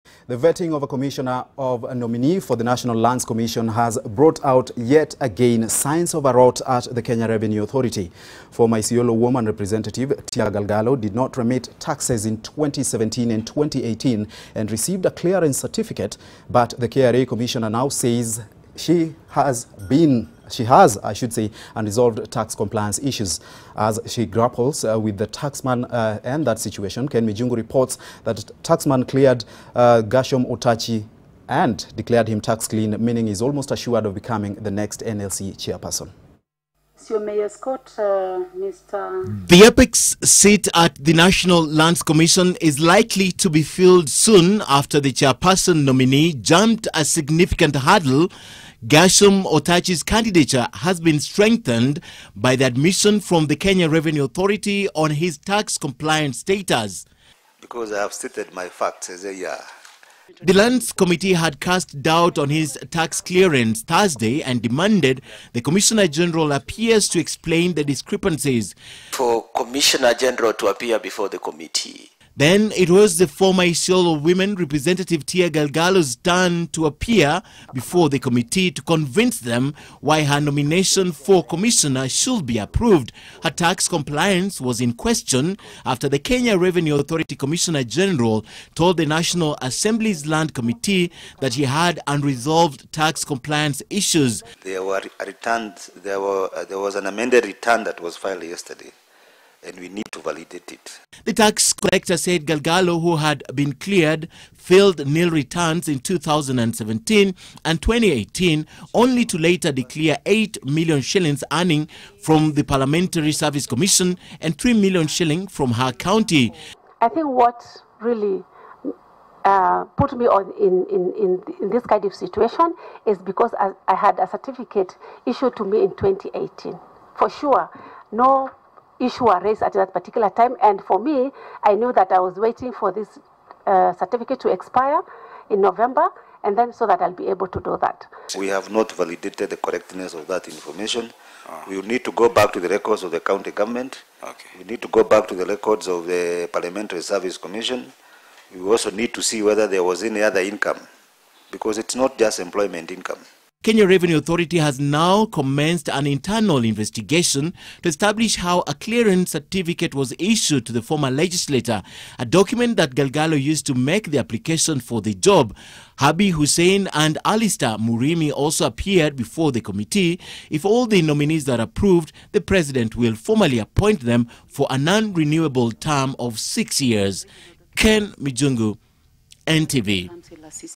KRA says Tia Galgalo is not tax clean despite being cleared
Gershom Otachi is only about to become the next NLC chairperson after the Kenya Revenue Authority clarified to the National Assembly’s Lands Committee that his tax compliance certificate was valid.